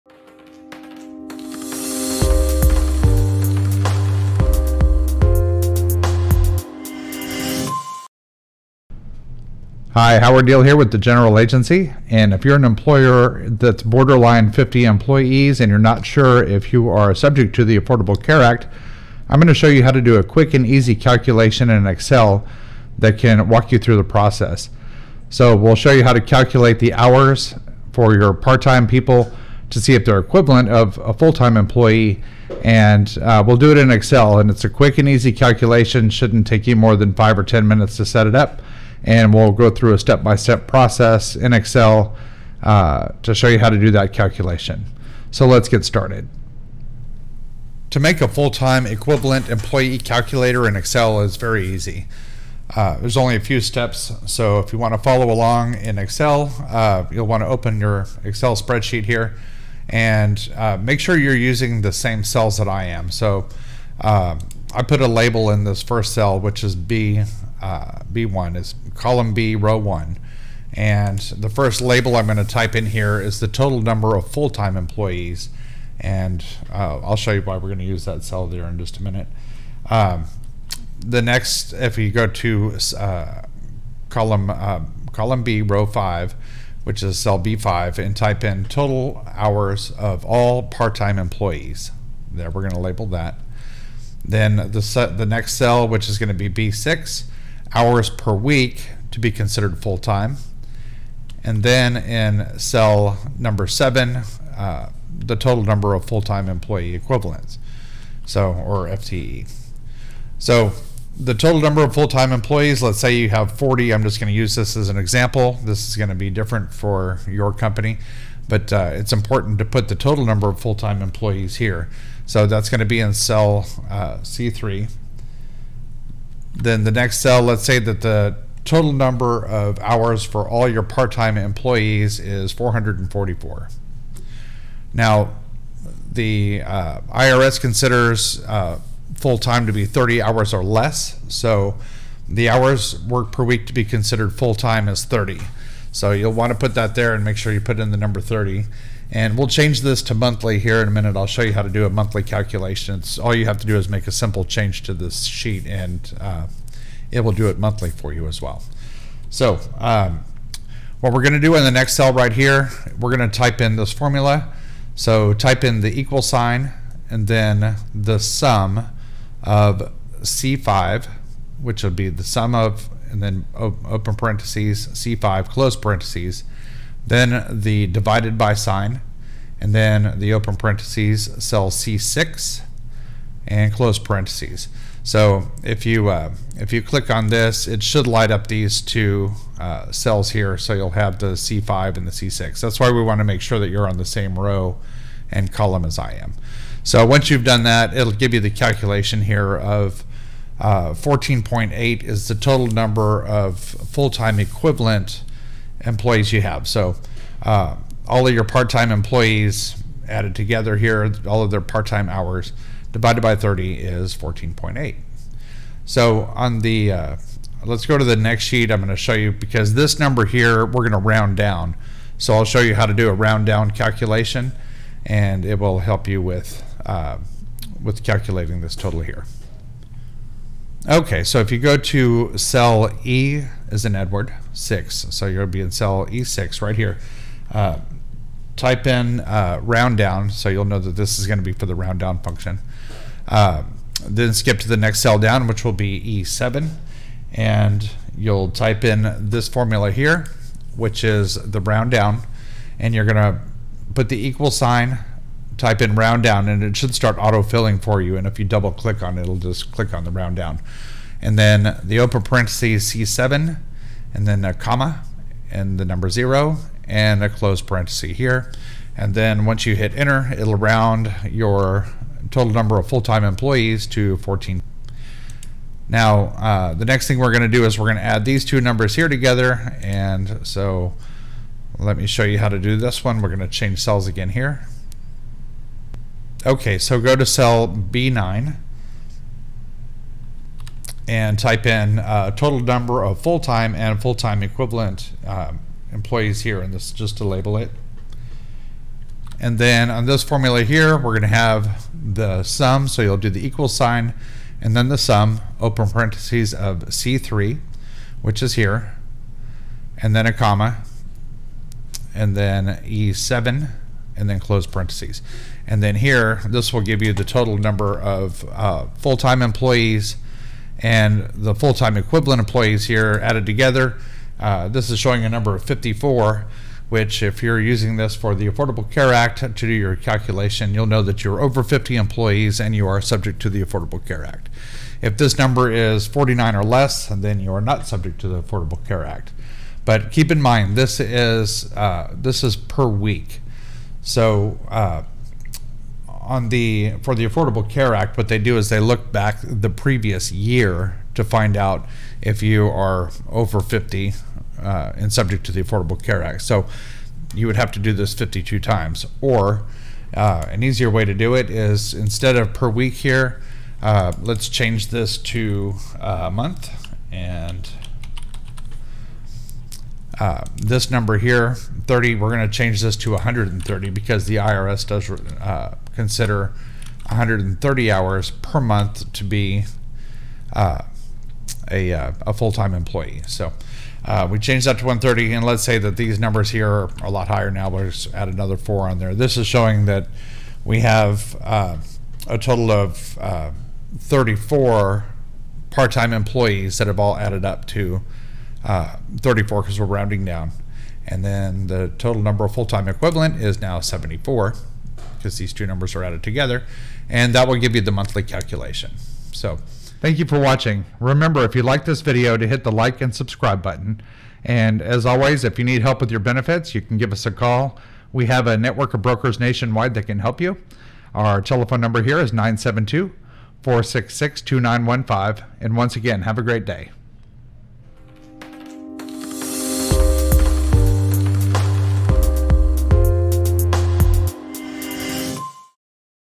ACA full-time equivalent employee (FTE) calculator in excel.
In this video we will show you how to calculate the full time equivalent (FTE) for the Affordable Care Act (ACA). The Affordable Care Act requires all groups with 50 or more employees to provide health insurance. Part time employees count towards the 50 employee threshold.

In other words, if you have 30 full time employees and 60 part time employees, the full time equivalent of your part time employees could put your group over 50 employees.

This is a step by step how to video which will show you the formulas to put in excel to do your calculations.